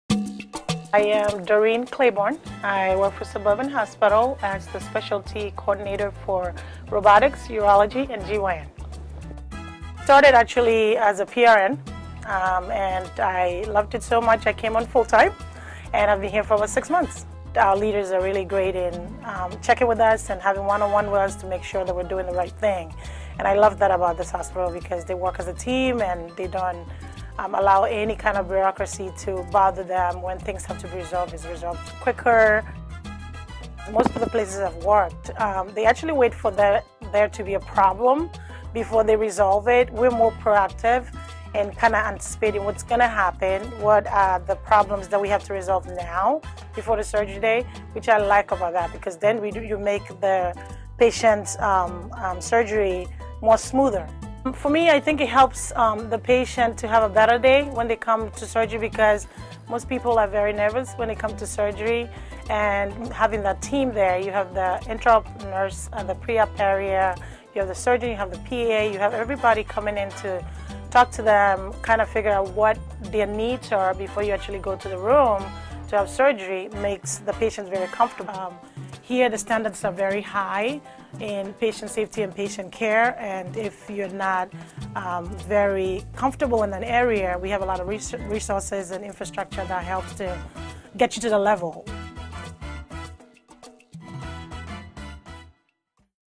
Interview: Doreen Clayborne, R.N.
In this video interview with one of Suburban Hospital's nurses, we discuss the role of nursing leadership, how nurses contribute to patient care, and more.